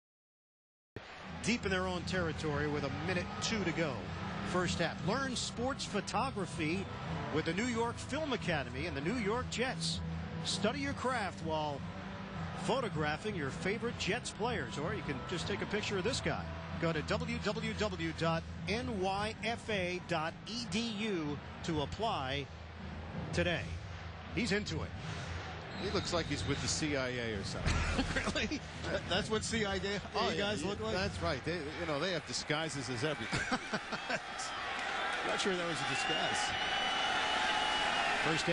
New York Film Academy Sports Photography on WCBS-TV During the New York Jets vs New York Giants Game
New York Film Academy sports photography student on the field during the New York Jets versus New York Giants exhibition game.